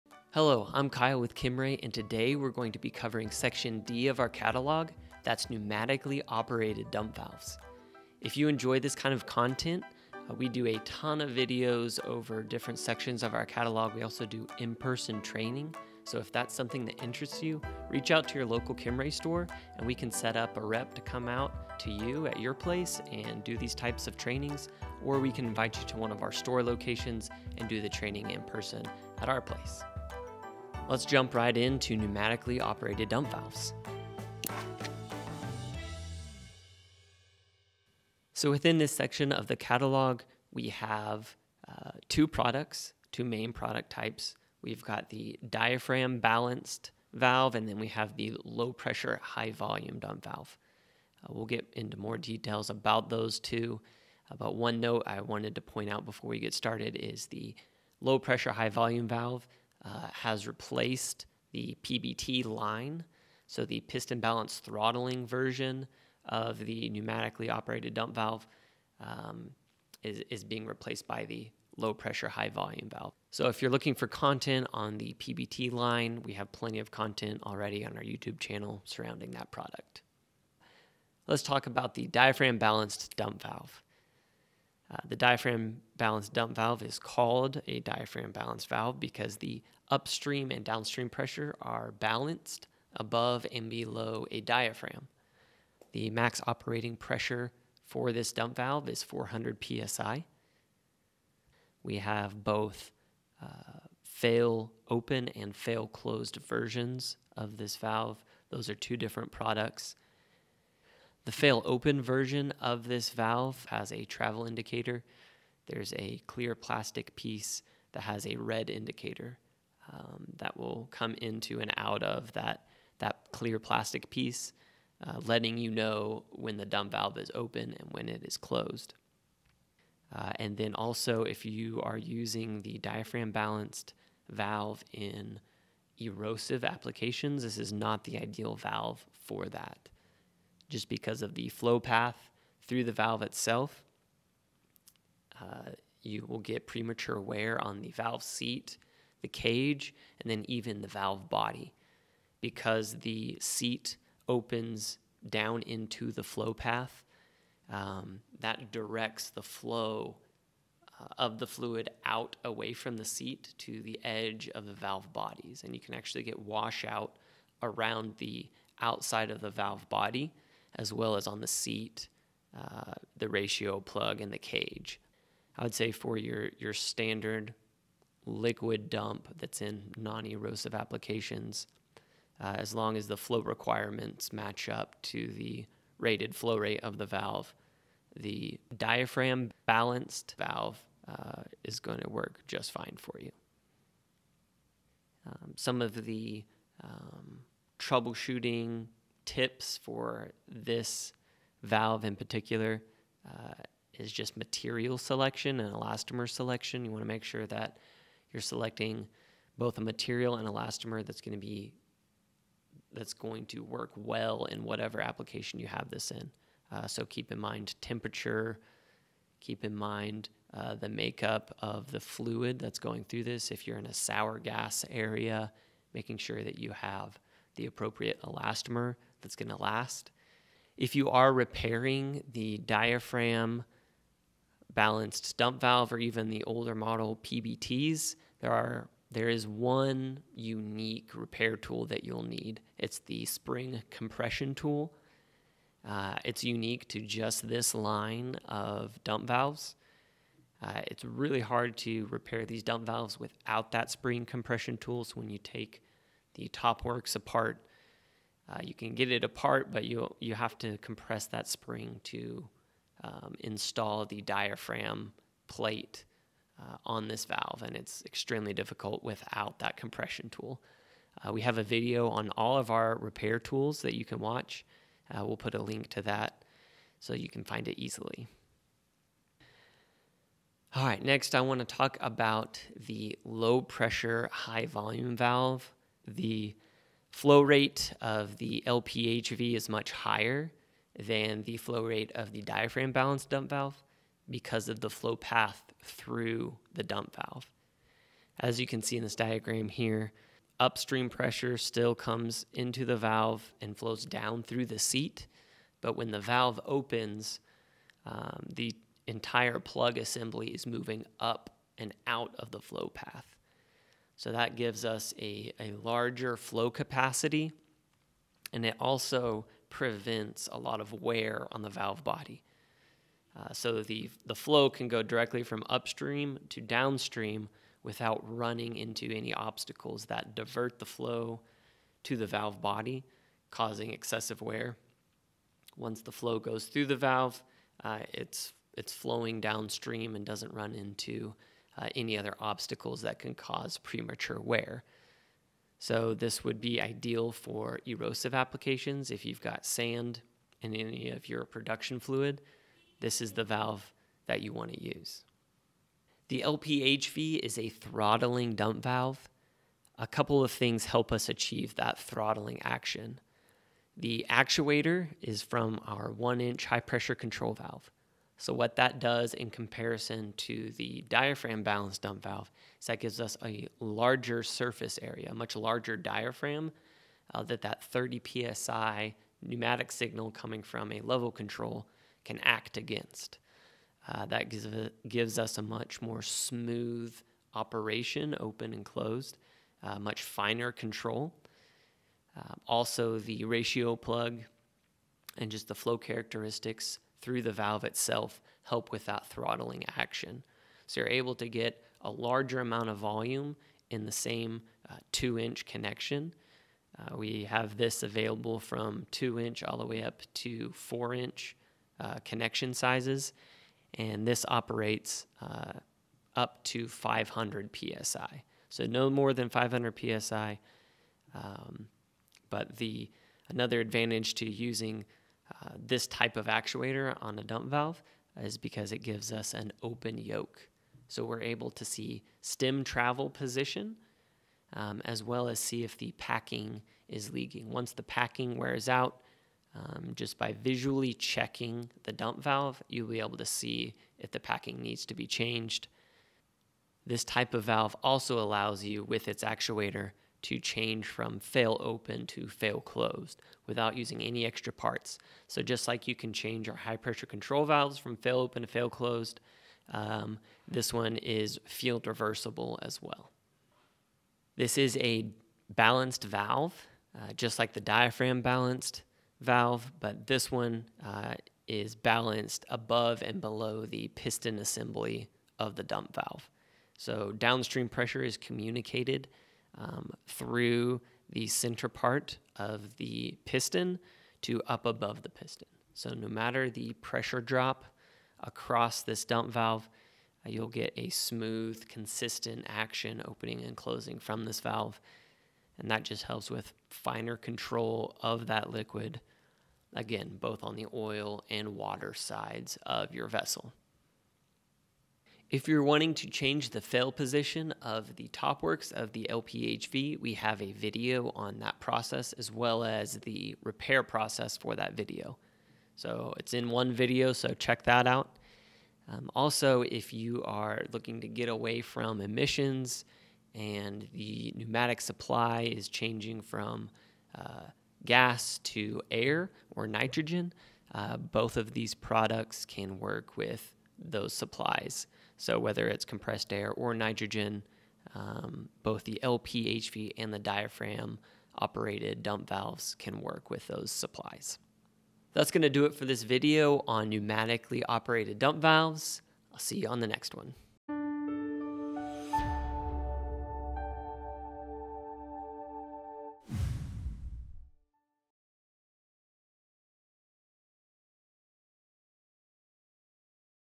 Pneumatically Operated Liquid Dump Valves (DB & LPHV) | Kimray Beyond the Basics Training
In this video, Kyle teaches through section D of the Kimray Product Catalog: Pneumatically Operated Liquid Dump Valves. This includes the Diaphragm Balanced Liquid Dump Valve and the Piston Balanced Throttling, Low Pressure High Volume Valve, or LPHV. We'll discuss flow paths and characteristics, applications, options, benefits, maintenance and more.

TIMESTAMPS:
0:00 Introduction
1:20 Diaphragm Balanced Dump Valve
4:40 Low Pressure High Volume (LPHV)